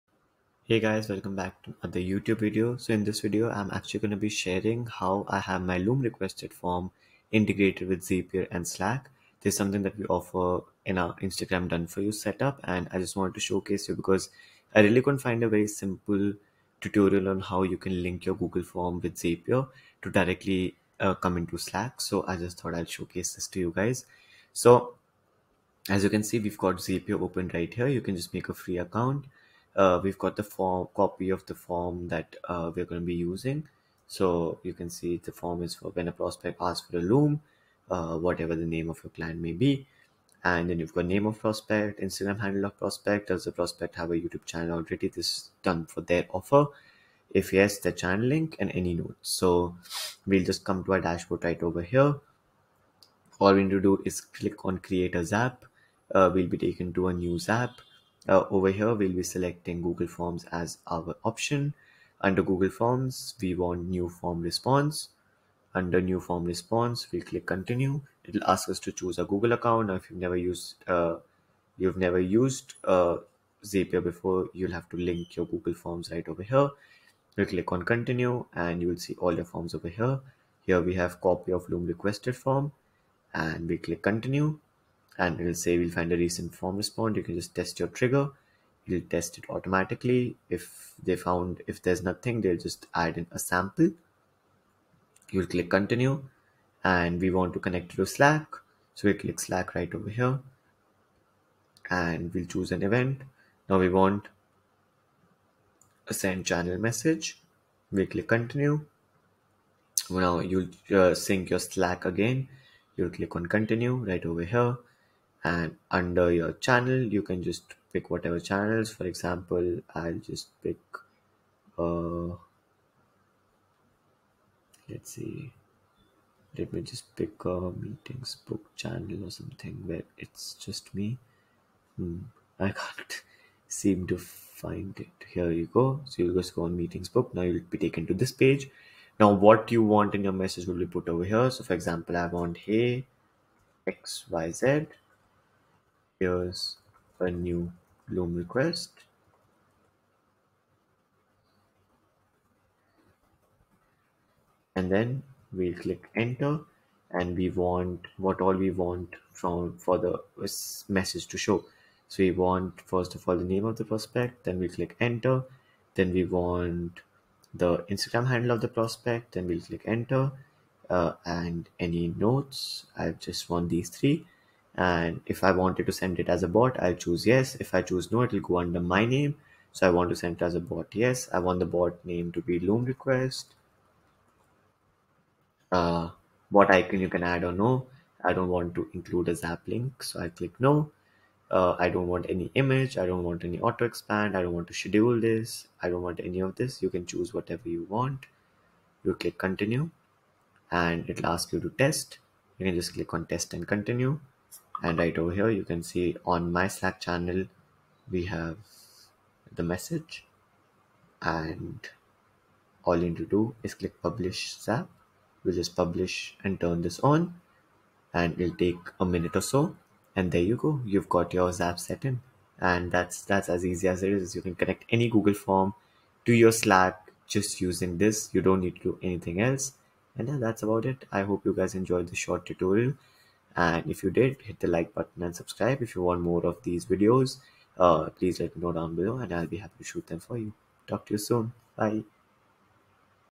How To Use Zapier To Automate Your Google Forms in 10 Minutes
Here's how I use Zapier to easily automate responses on my Google Forms for our DFY IG Outreach!

To buy freshly sourced leads and lines according to your needs